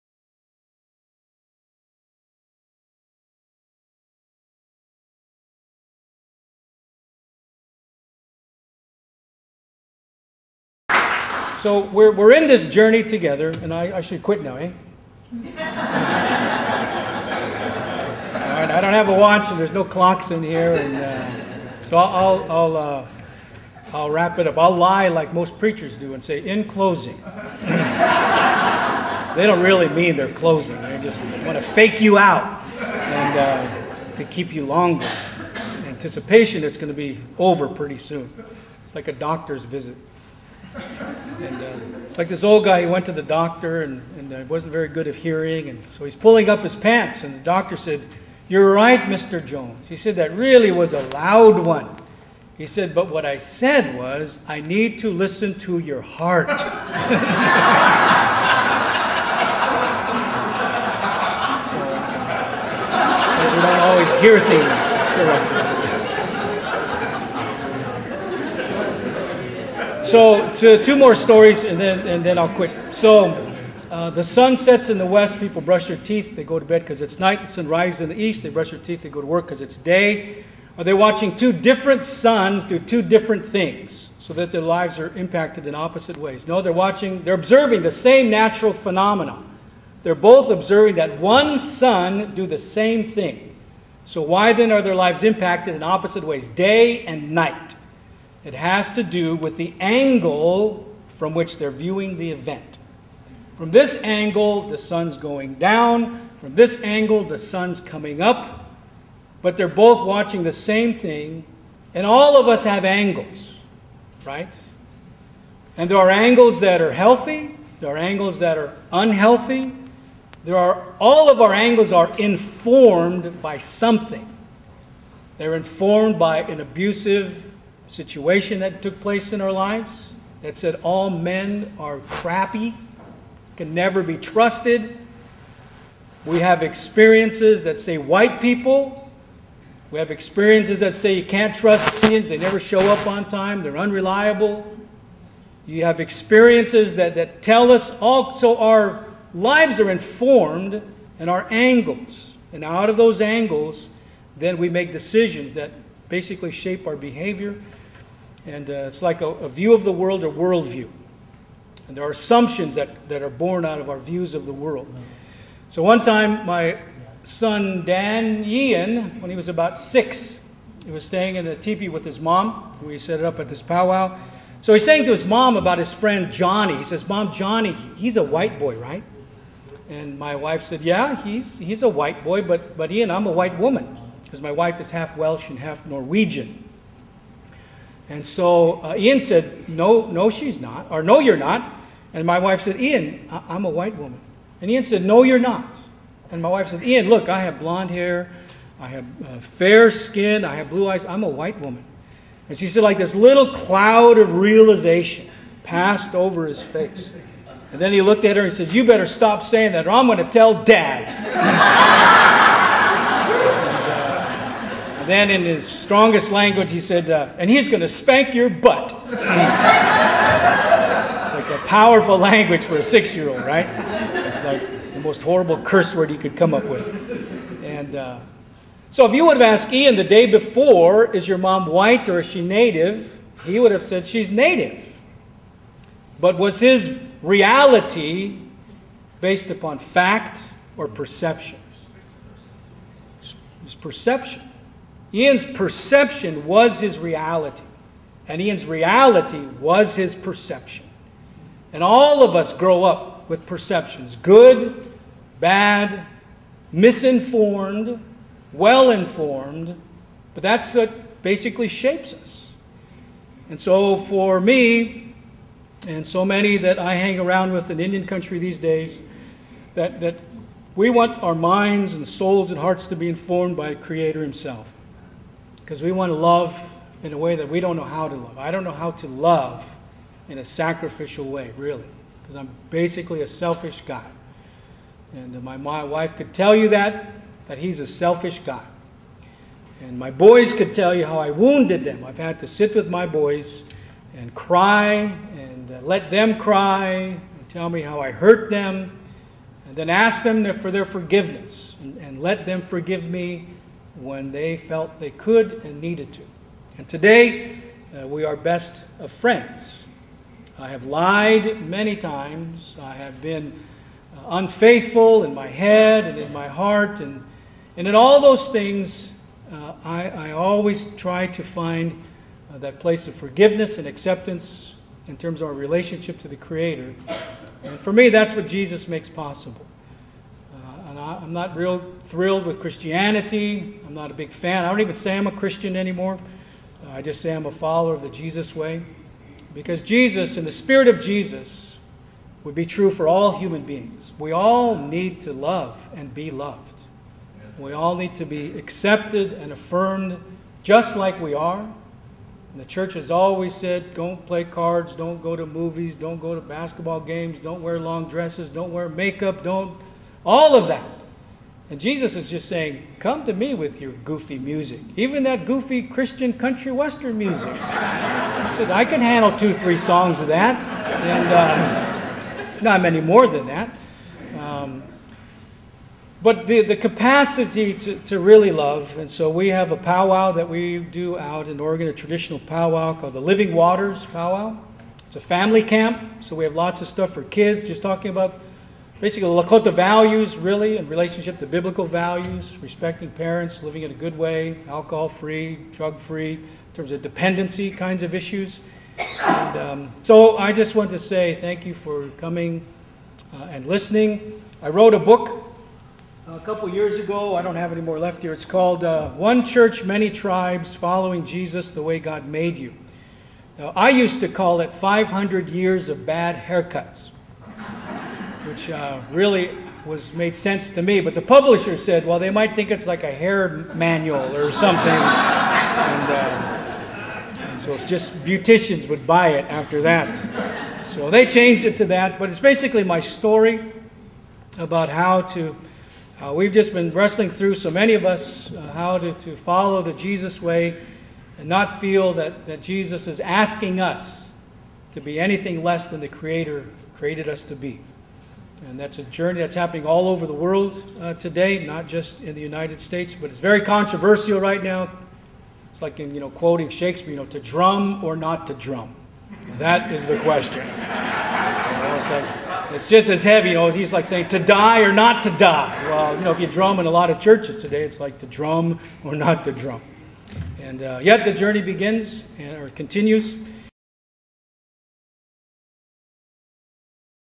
Richard Twiss 5
"To Live in a Good Way" ...a talk by Richard Twiss (Rosebud Lakota)
Hosted by United Tribes Technical College